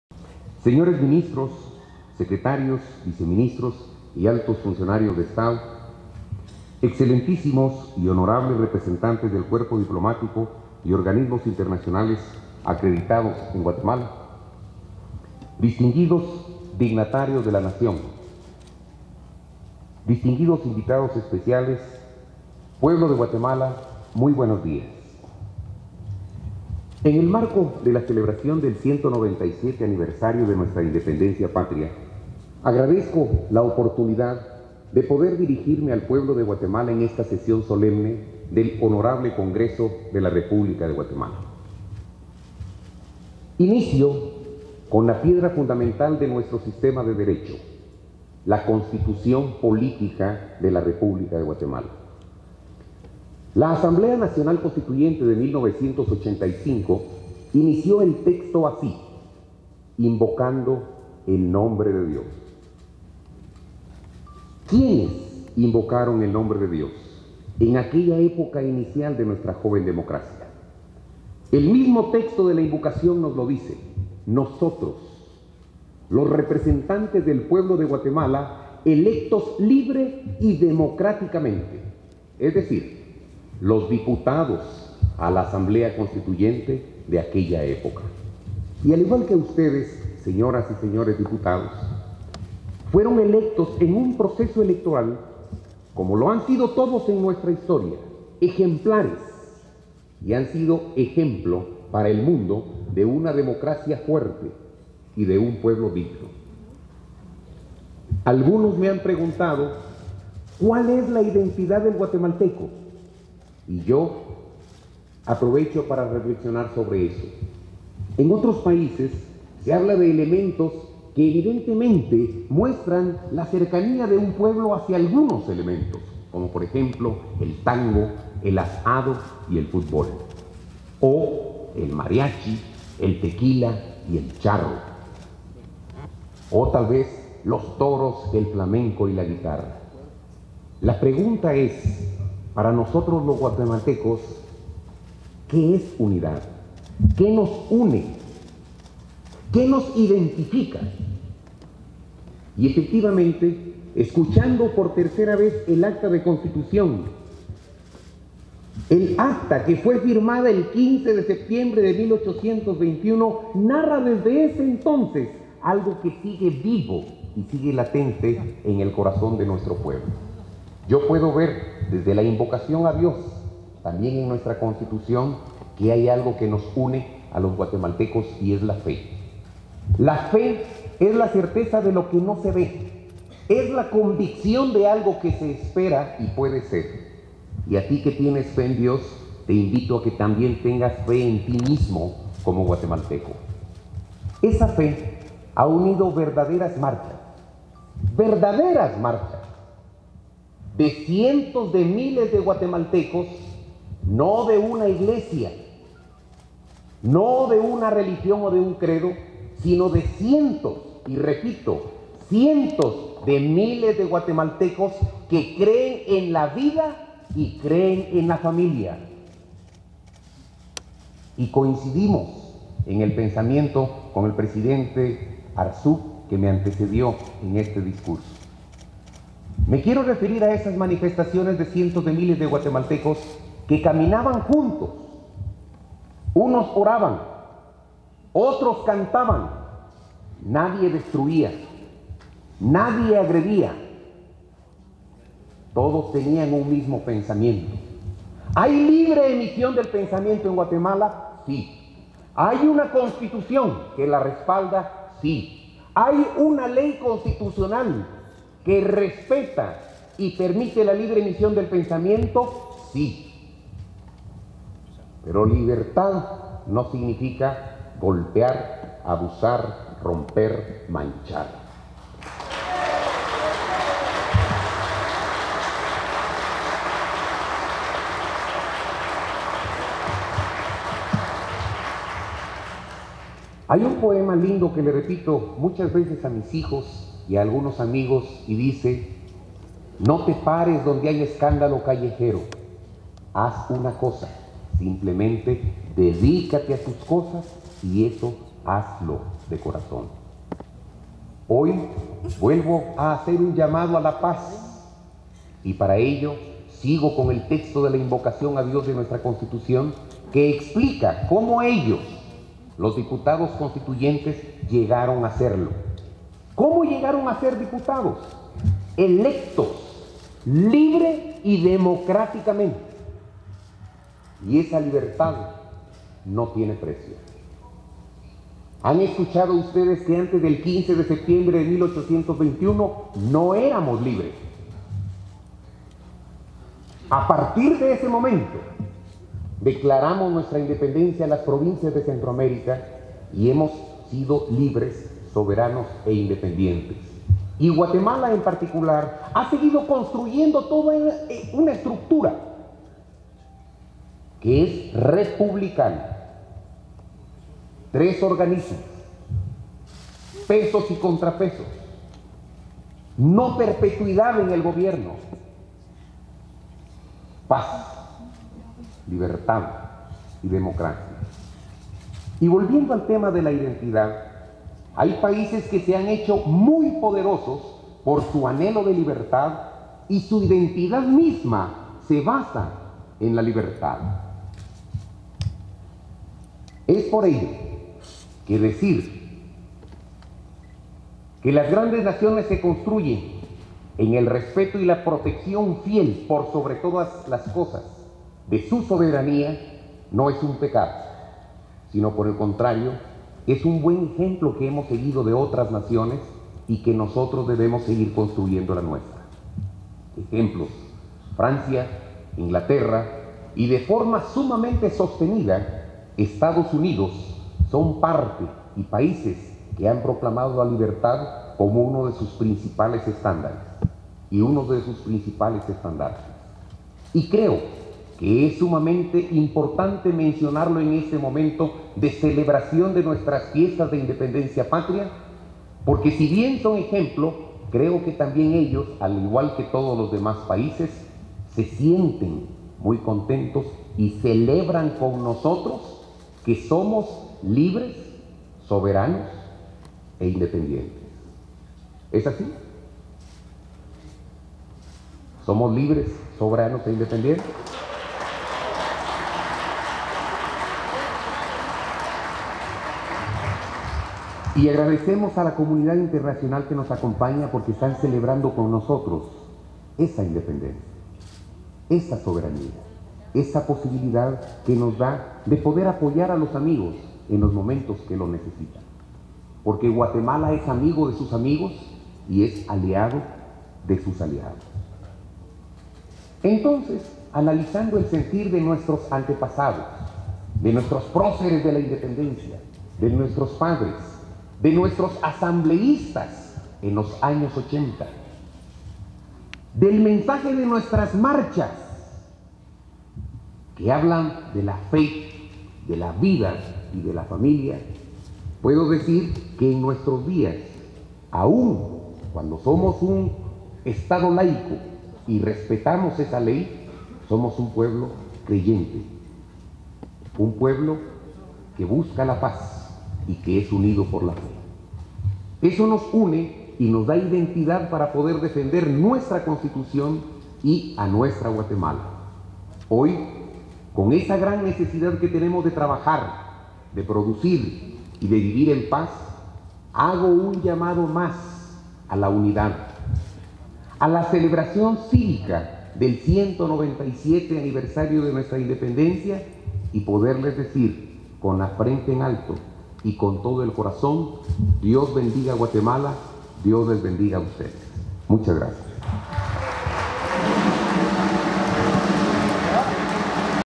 Presidente insta al respeto y protección de la soberanía
En el marco de la sesión solemne por el CXCVII Aniversario de Independencia de Guatemala, celebrada hoy en el Congreso de la República, el presidente Jimmy Morales instó a los guatemaltecos a respetar y proteger la soberanía del país, como “lo hacen las grandes naciones”, argumentó, en tanto celebró la búsqueda de la paz por parte del pueblo.

    “Las grandes naciones se construyen en el respeto y la protección fiel, por sobre todas las cosas, a su soberanía”, mencionó Morales, agregando que países como Francia, Inglaterra y Estados Unidos “han proclamado la libertad como su principal estandarte”.

Así, el mandatario reflexionó sobre la identidad de la nación, la cual, a su criterio, se funda en la fe de los guatemaltecos.

Además, recordó que la Constitución Política garantiza la libre emisión del pensamiento, sin embargo, “esta libertad no significa golpear, abusar, romper o manchar”, remarcó.

El gobernante finalizó haciendo un llamando a la unidad y a desarrollar la celebración de la independencia de forma cívica.